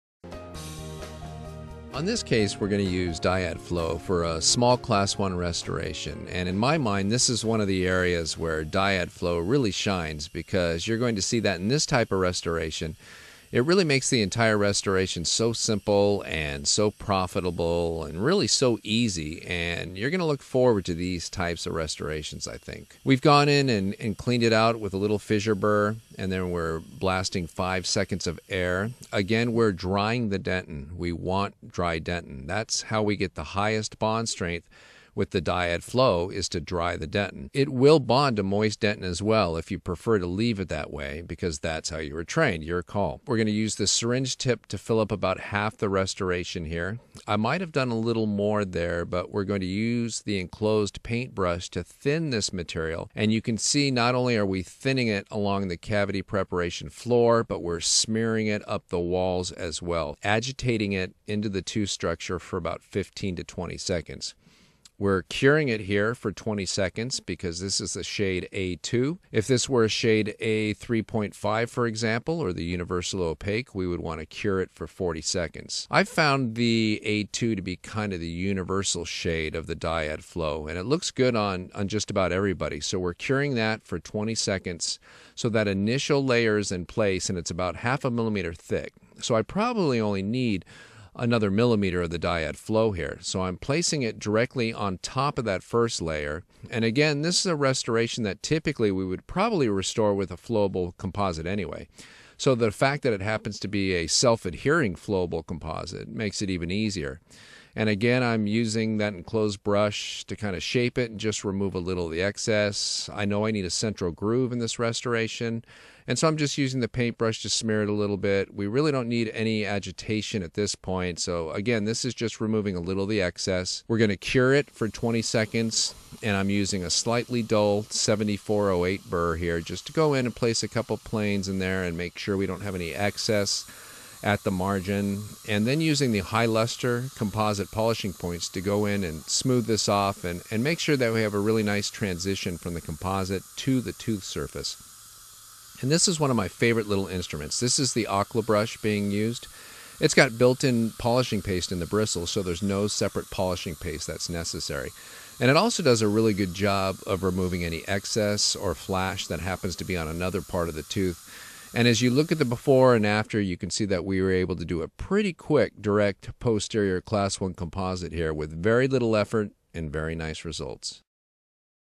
Dyad Flow Restauración Clase I from Kerr Corporation
Dyad™ Flow simplifica los procedimientos en sus restauraciones
directas, al incorporar un agente adhesivo en la resina fluida, la cual
está basada en la reconocida tecnología adhesiva de OptiBond de
Kerr. Nuestro liderazgo en odontología restaurativa, viene luego de
décadas de avances en resinas compuestas y nuestra experiencia
con adhesivos dentales -- ambos aspectos se unen en Dyad Flow